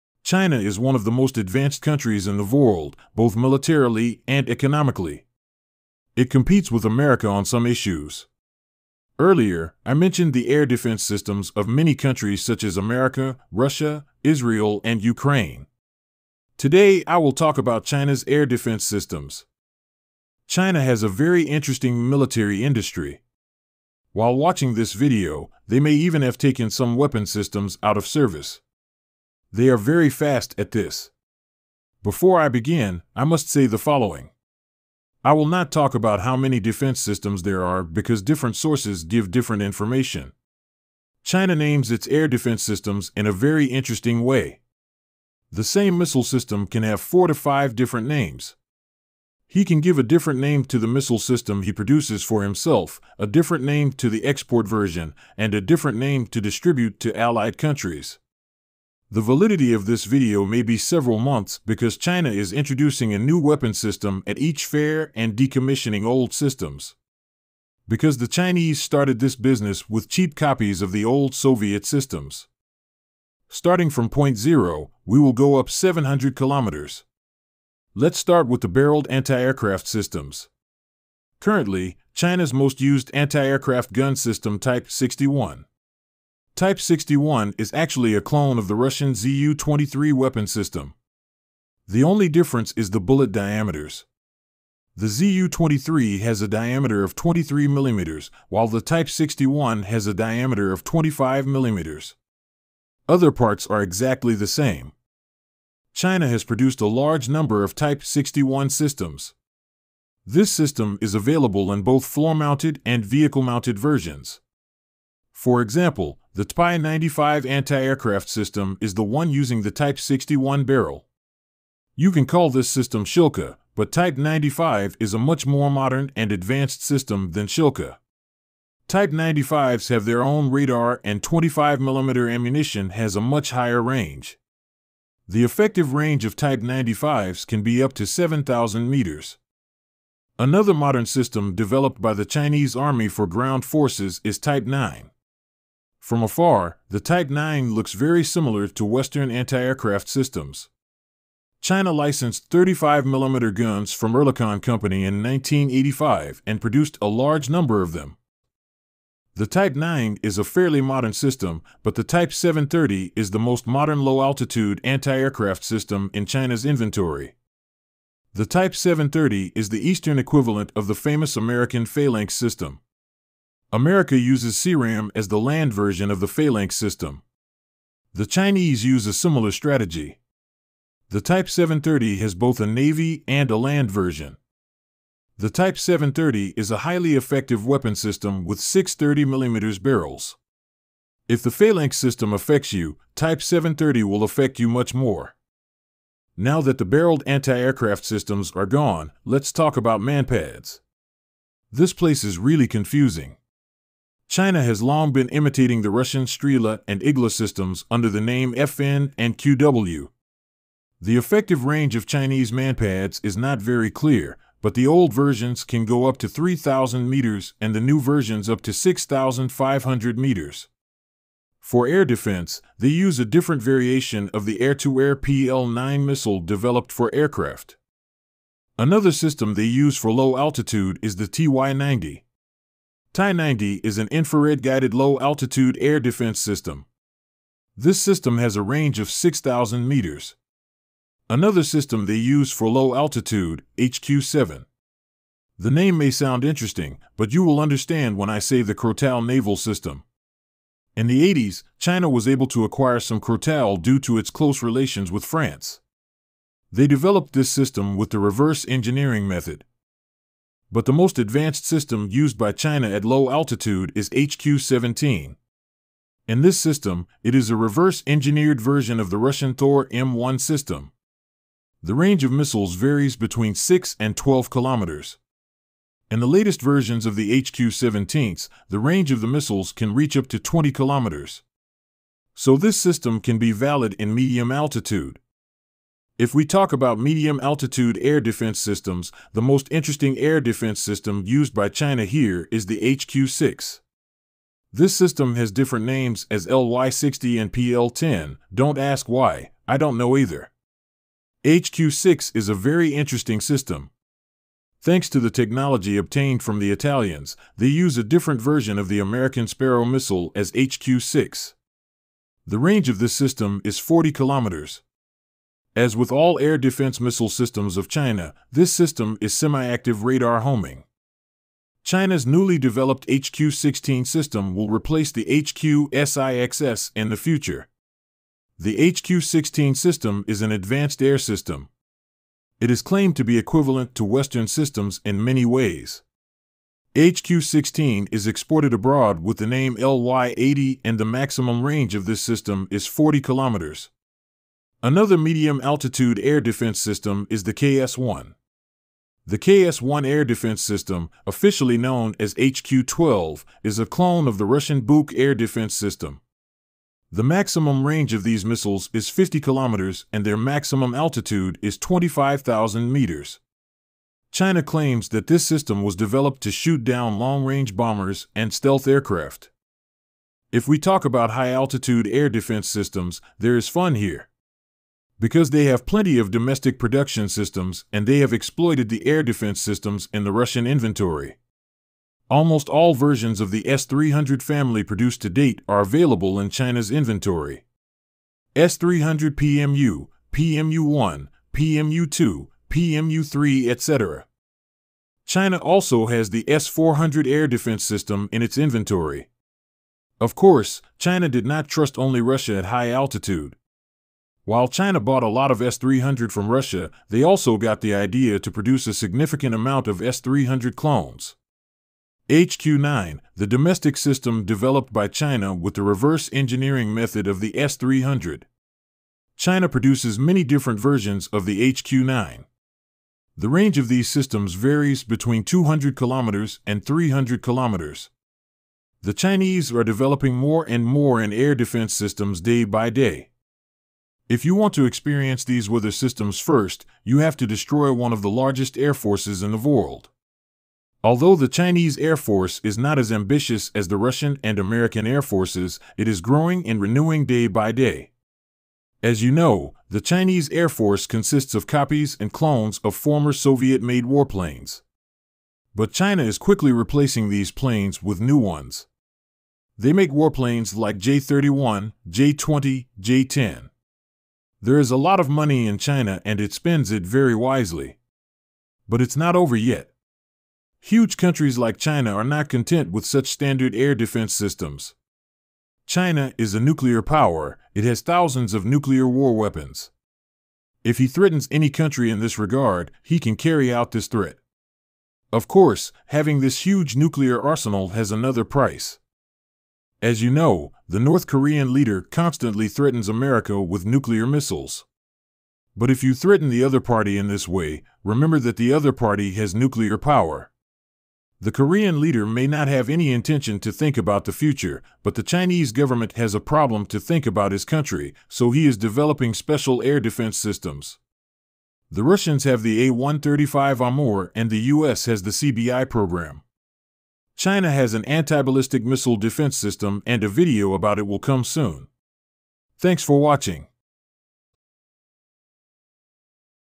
China's air defense systems.
In this video, I talked about China's air defense systems. If you found the video useful, you can support it by liking it.